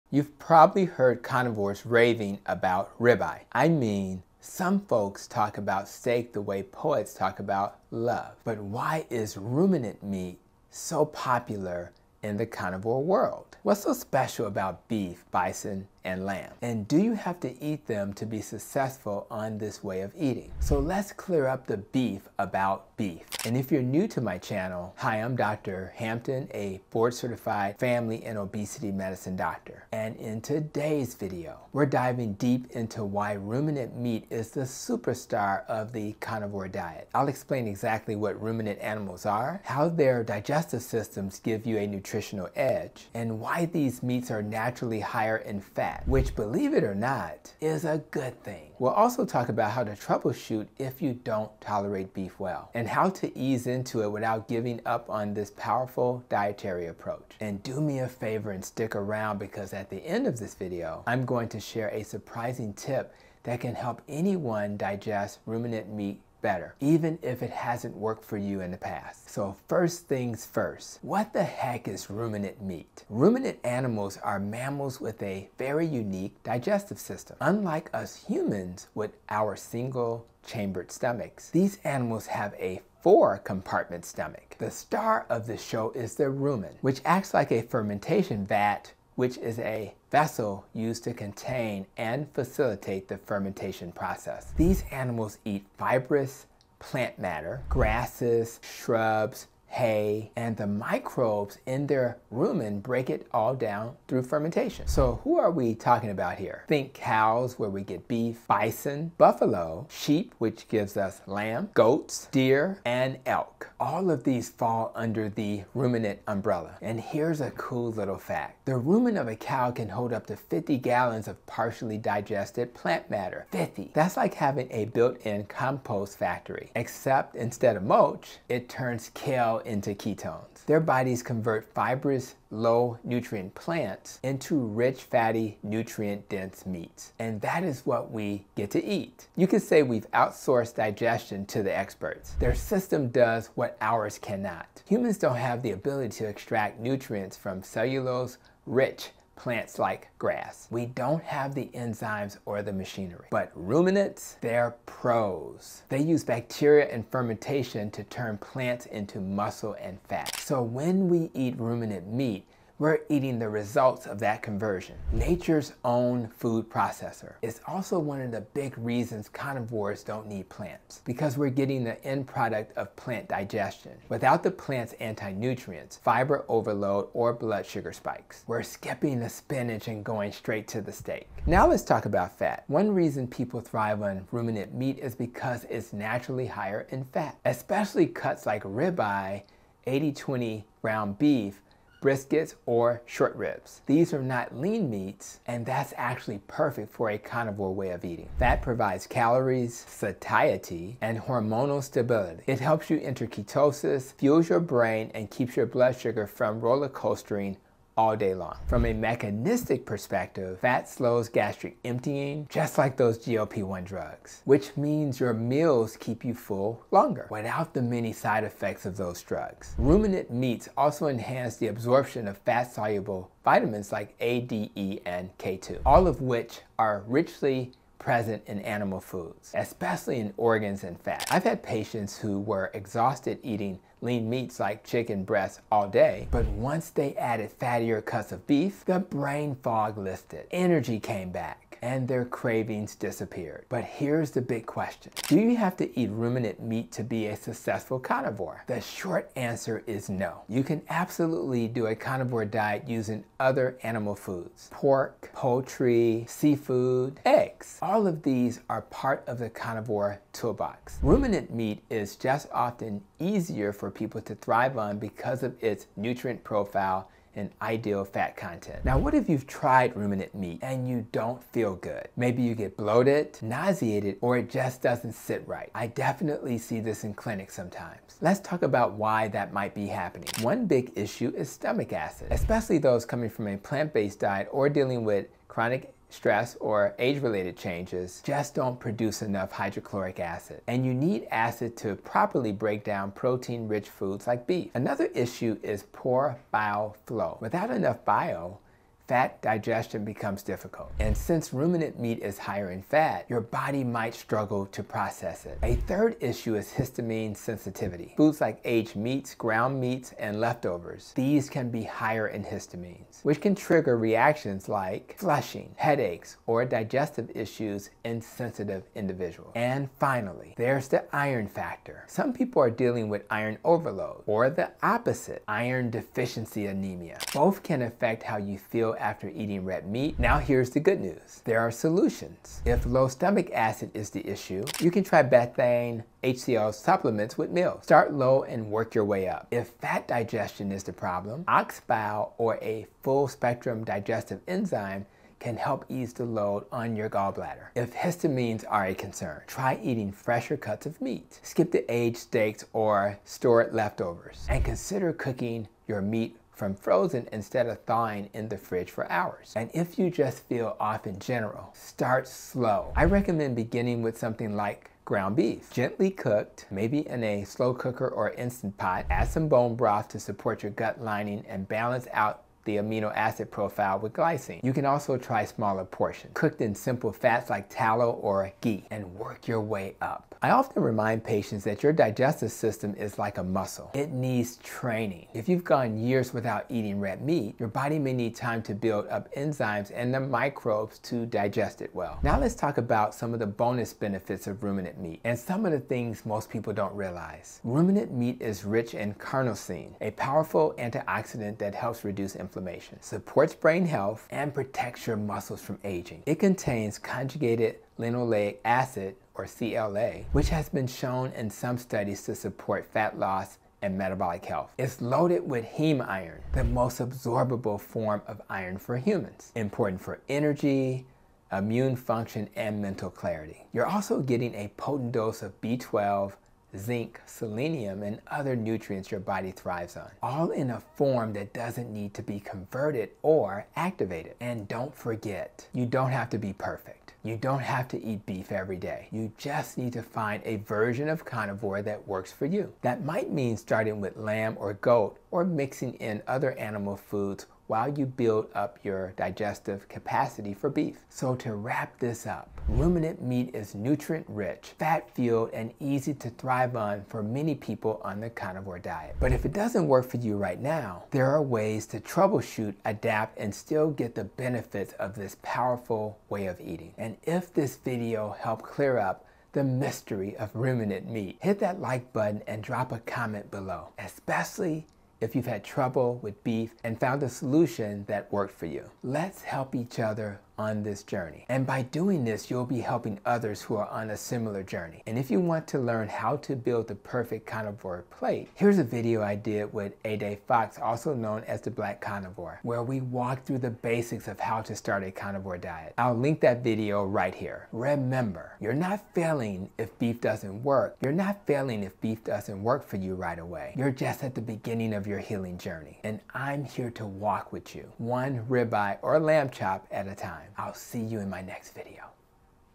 Why Carnivores Prefer Ruminant Meat (And Should You?)
Why do carnivores go crazy for ruminant meat like beef, lamb, and bison? What makes it so special—and do you have to eat it to thrive on a carnivore diet? In this video, Dr. Tony Hampton breaks down everything you need to know about ruminant animals, their unique digestion, and how they turn grass into one of the most powerful foods on earth.

You’ll learn:

What ruminant meat is and which animals qualify

How their four-chambered stomachs do the “plant digestion” for us

Why ruminant meat is richer in fat and nutrients

How fat helps with energy, hunger, and hormonal balance

What to do if you struggle to digest beef

Tips to overcome issues like low stomach acid, bile problems, or histamine sensitivity

And why carnivore doesn’t have to mean eating beef every day

Whether you're new to the carnivore diet or troubleshooting your results, this is the video you need to unlock the full power of animal-based nutrition.

00:00 Carnivores Prefer This: Why Ruminant Meat Is King
01:11 What Is Ruminant Meat?
01:44 Which Animals Are Ruminants?
02:10 Why Carnivores Don’t Need Plants
04:04 Do you have to eat ruminant animals
04:28 What If You Can’t Digest Red Meat?